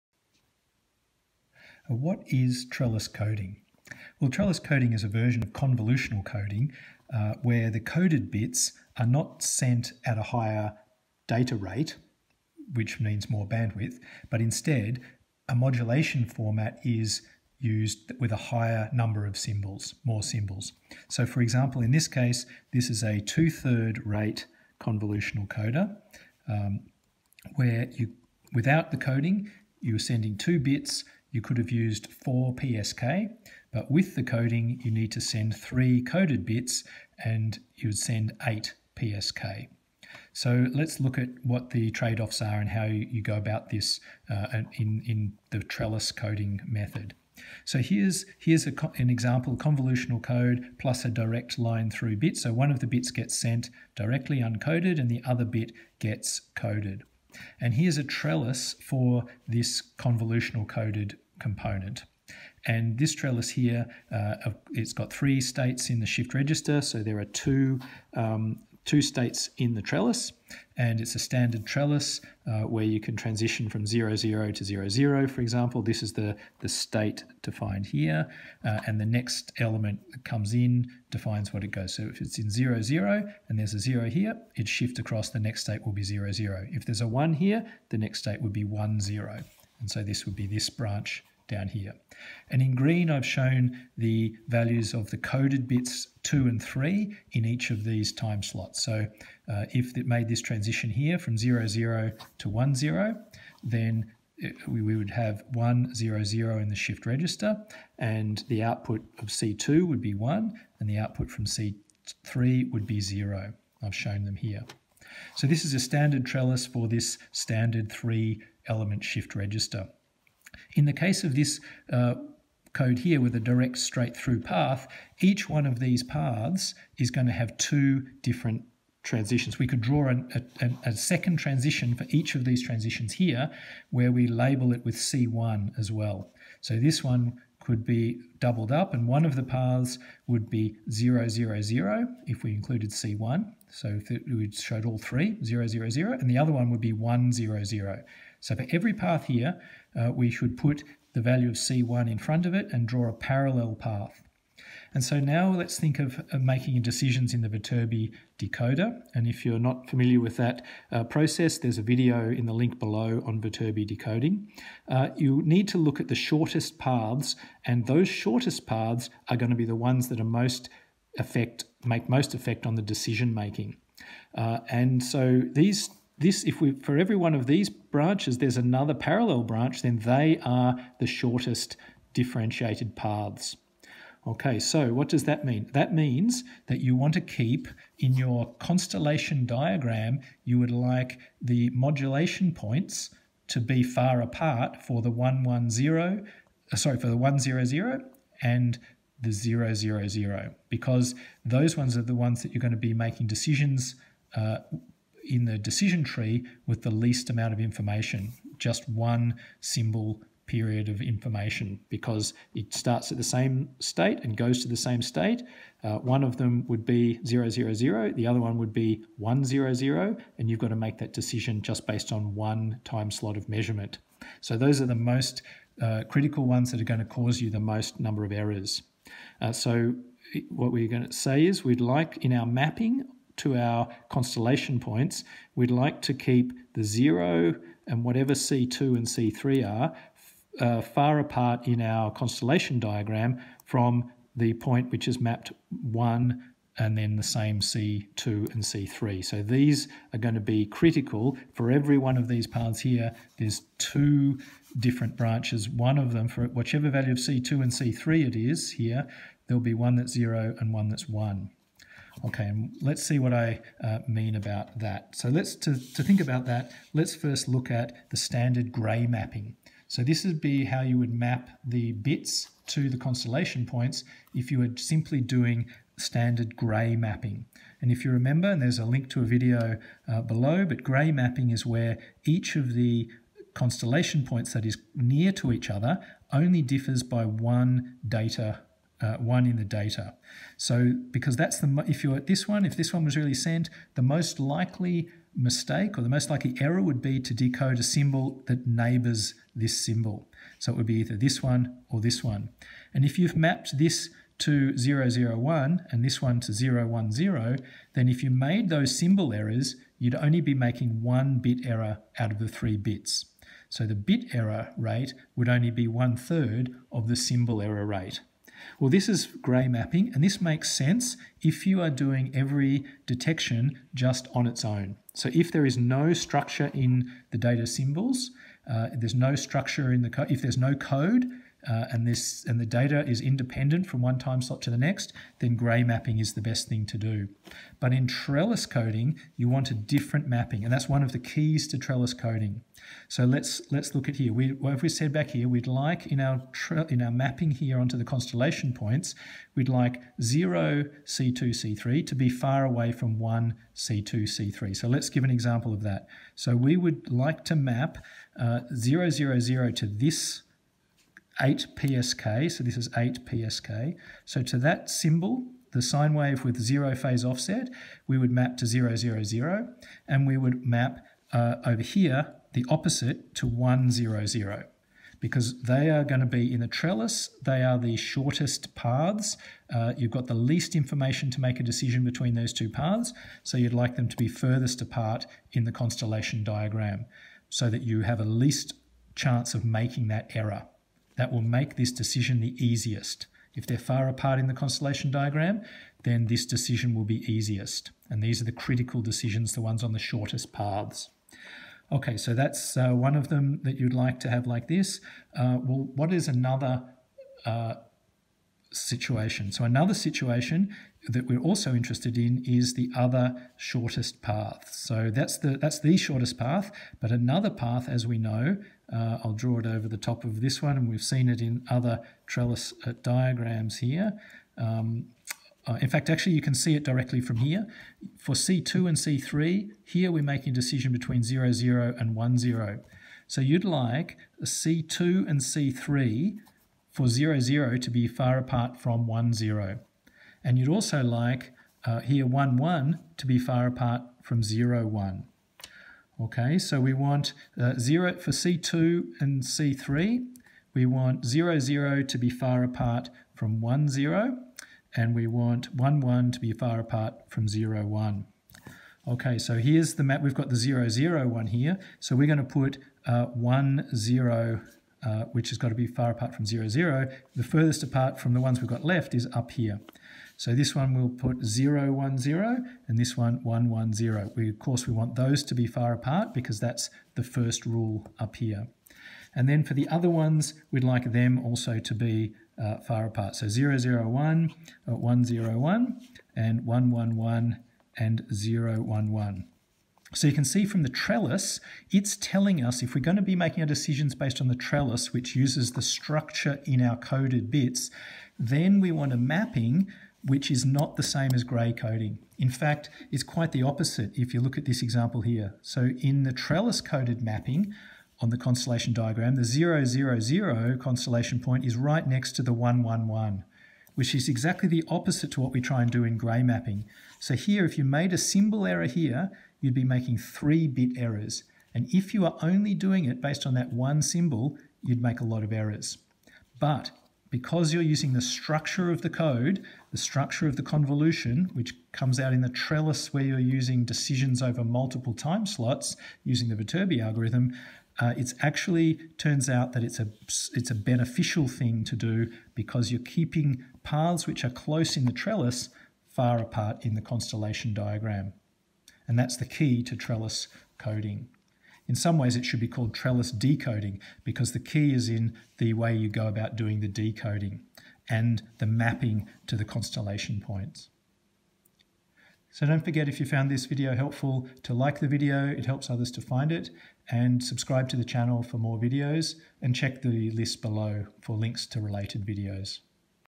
What is Trellis Coding?
Explains Trellis Coding using a basic example.